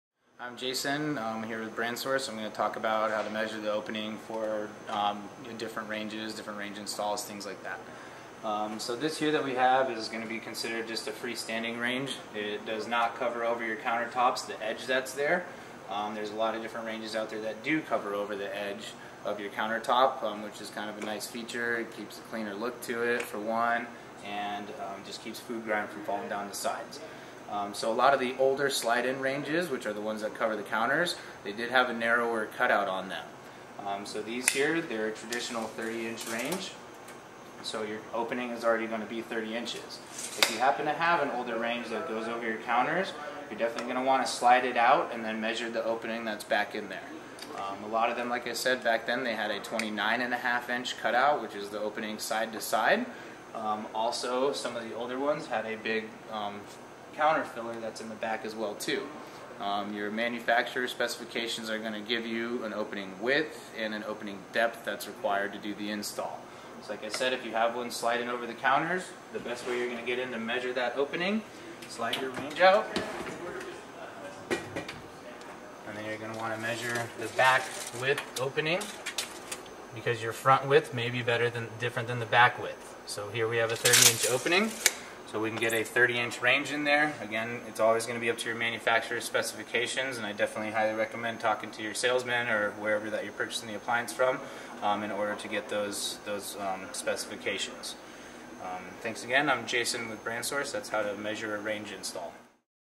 How To Measure Your Space for a Range Install
Jason shows us the easiest way to measure your kitchen space for a new range or oven. Make sure you get accurate measurements so you have a simple and stress free installation!